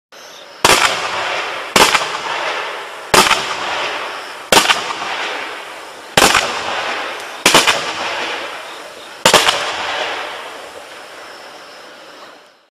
Bronzefächer (BX-6810)
★ FWA Fächerbatterie: Bronzefächer ★

Prächtige Fächer aus Titan-Gold Feuertöpfen. Abschuss im 7er Fächer (stiller Effekt)